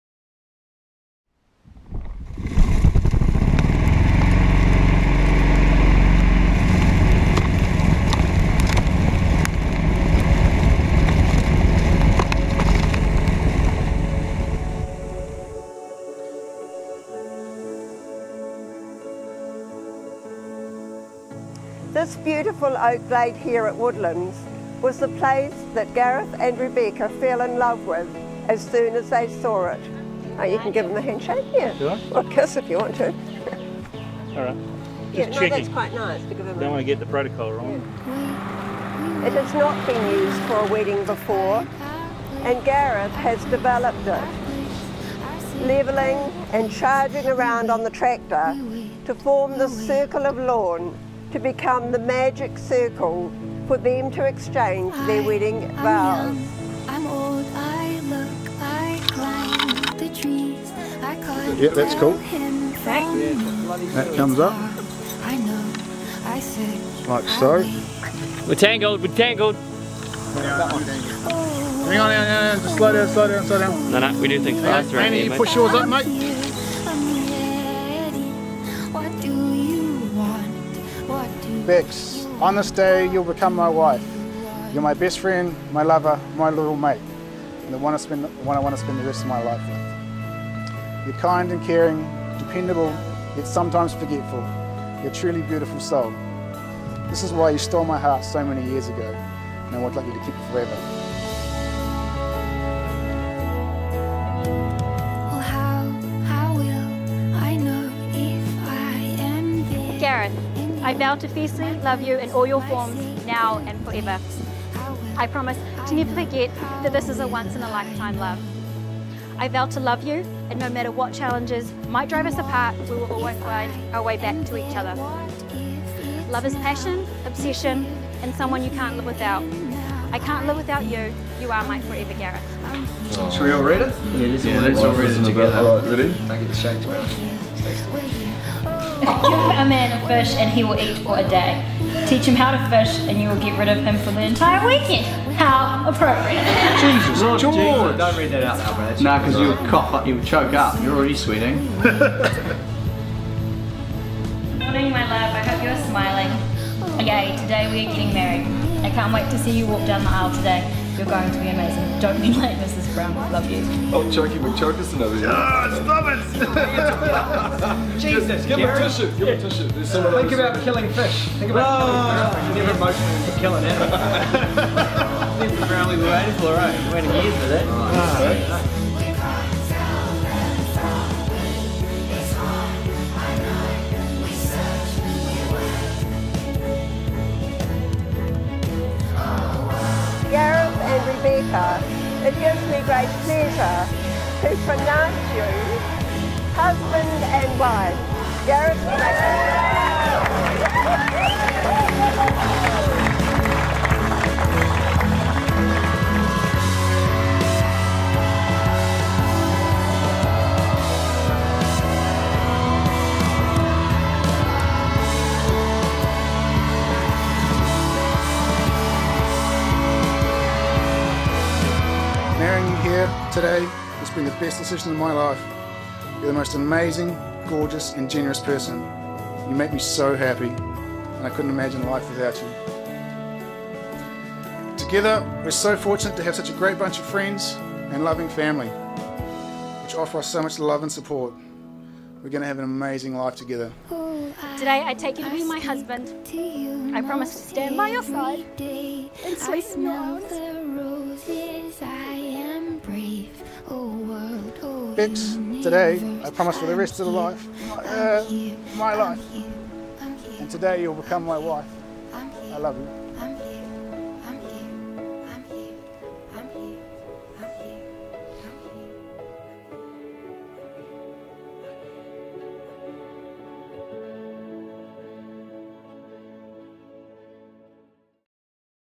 Married in a FOREST - Gareth + Bex
For more unique videos like this, check out perspectives.co.nz  (because forest weddings are cool!)

They say every thundercloud has a silver lining – sometimes when “plan A” changes dramatically, you realise that it all happened for a reason.

Bex and Gareth originally planned to marry in Auckland, but when those plans needed to change, they instantly fell in love with Woodlands. But not just the traditional parts of this beautiful venue – Gareth (a landscape architect by trade) saw the potential in the beautiful grove of oak trees out back – so over the next year they and their support crew groomed and sculpted it into exactly what they wanted.

Of course, you can’t plan everything – and despite all their finger crossing and breath holding – the rain arrived.
The silver lining? Rain photos galore, the opportunity for some beautiful photos inside the homestead and a little gift of the most perfect patch of sunshine just as they entered their reception.

Forest Weddings don't come without a little effort and planning.  Here's one that didn't.  A Westie No Rules Wedding.